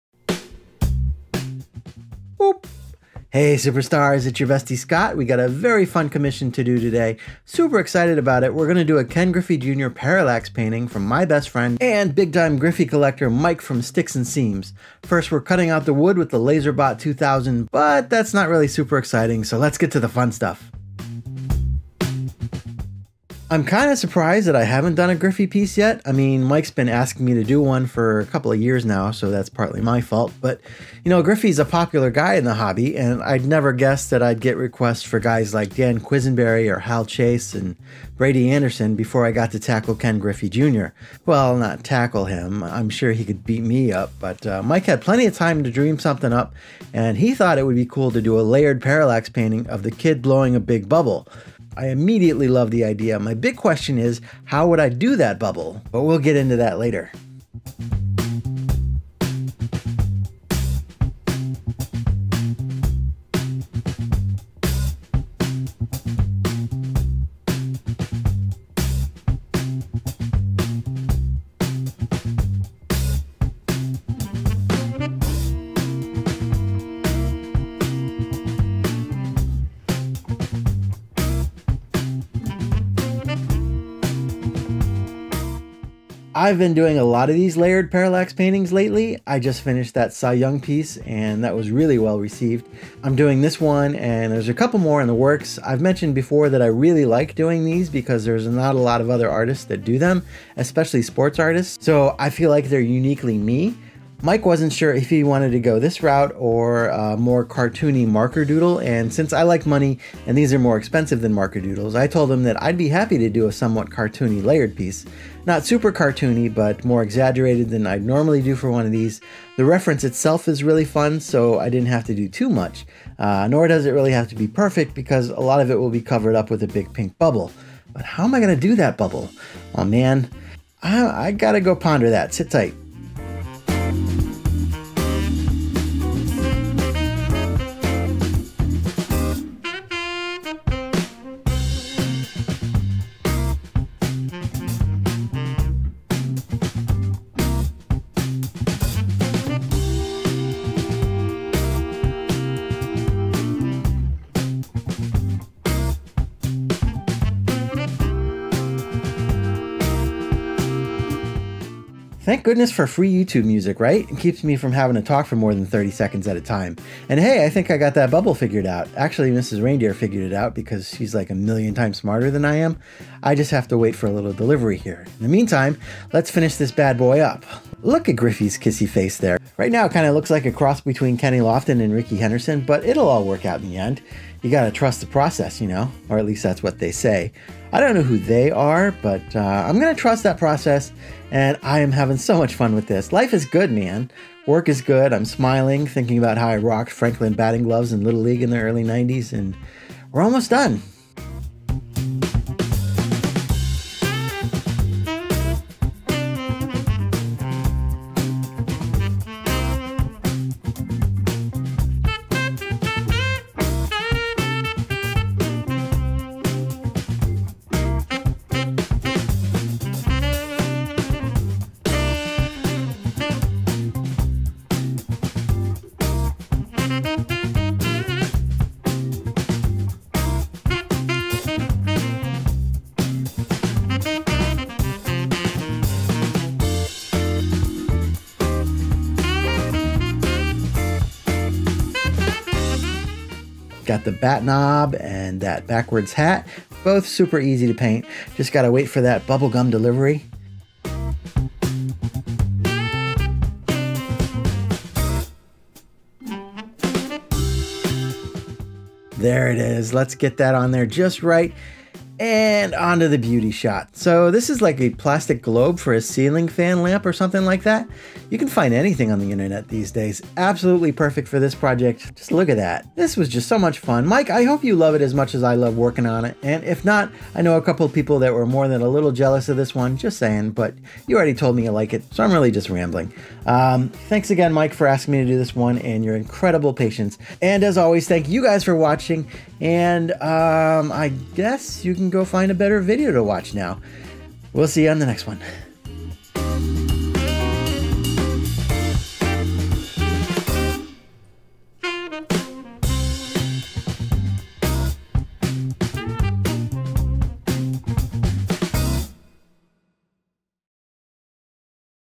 Ken Griffey Jr. Bubble Boy Parallax Painting
In this video, I paint a 3D parallax painting of Ken Griffey Jr. blowing a bubble for my best friend, Mike at @sticks_n_seams9904 .

If you’d like to support Reindeer Studios, please consider buying something from my Etsy store: reindeerstudiosstore.etsy.com